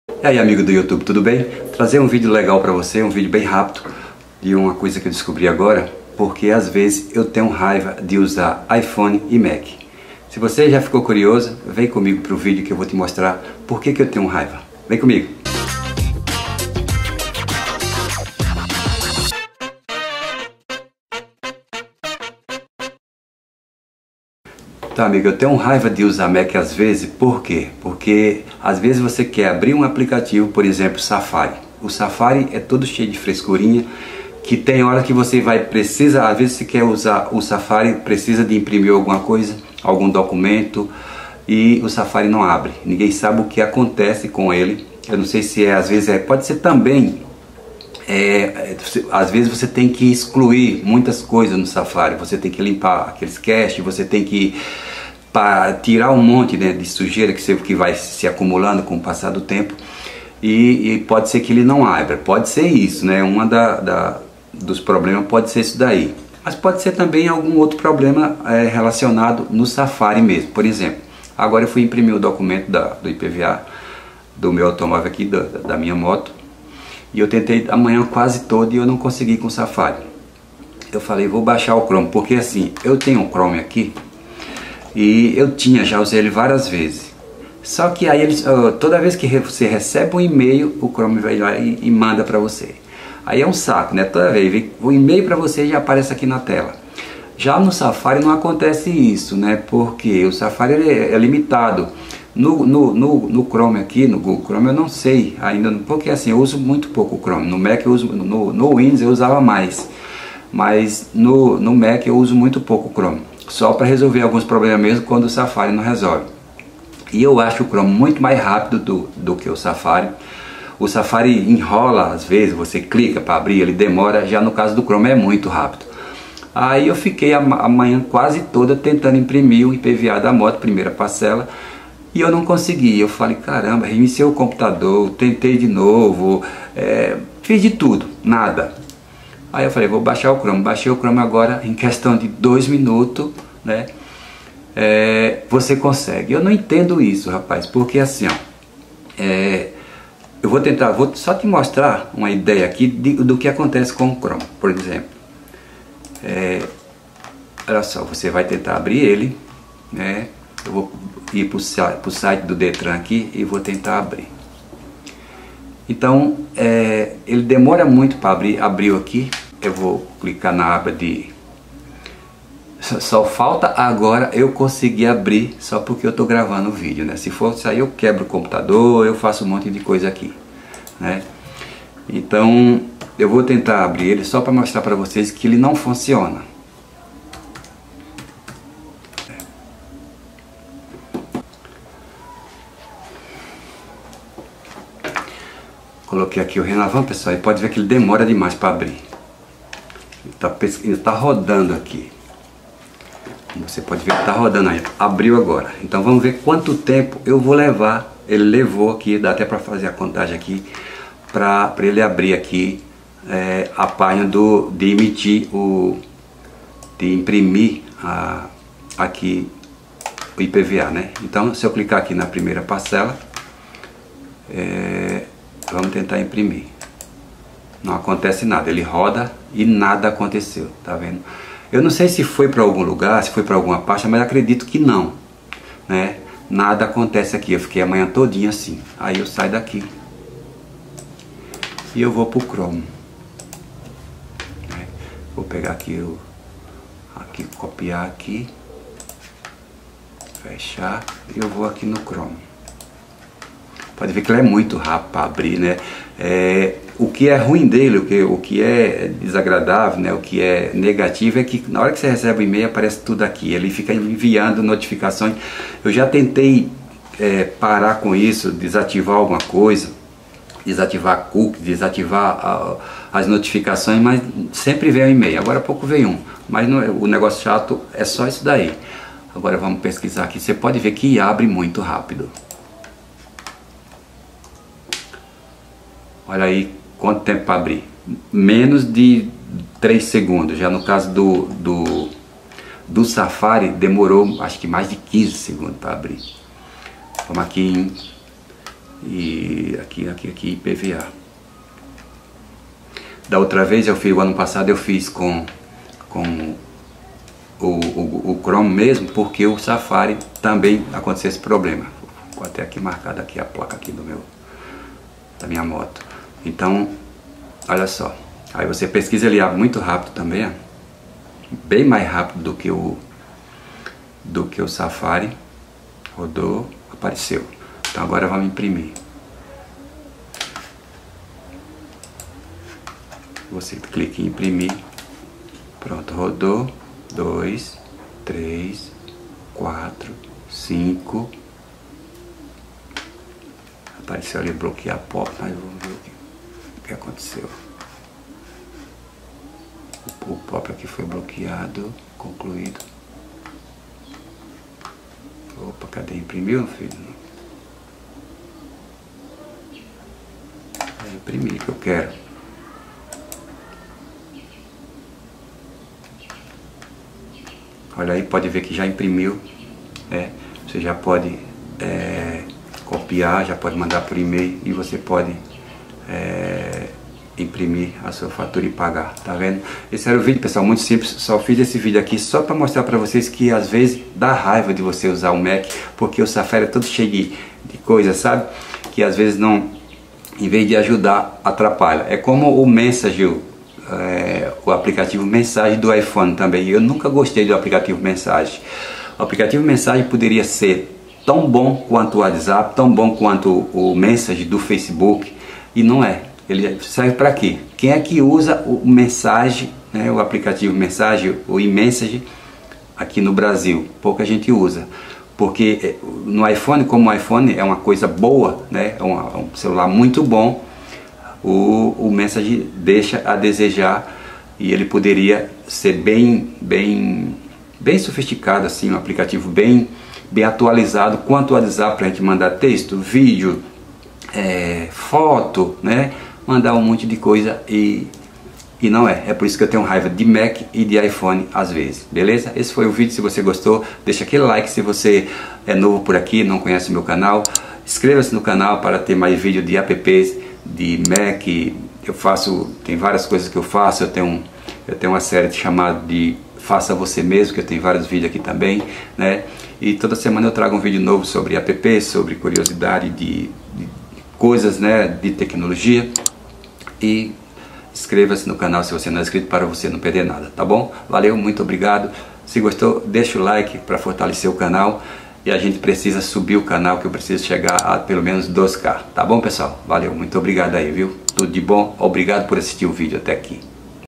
GOOGLE CHROME DEIXA SAFARI COMENDO POEIRA
FAÇA UMA DOAÇÃO, PRECISO MUITO DE SUA AJUDA

AG: 4155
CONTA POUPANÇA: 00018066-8
OP:013
PEDRO SILVA OLIVEIRA

Nesse vídeo vou te falar o porque as vezes da raiva de usar o Safari, quando a gente mais precisa ele dá pau e não abre. Veja porque nesse caso preferi o Google Chrome, muito rápido, só não é o melhor porque fica mandando  um monte de notificações. Nesse caso ele é chato porque fica mandando notificações o tempo todo. Se caso você souber desativar deixe comentários aqui embaixo.

Muito obrigado por está no meu canal! Valeu! Grande abraço!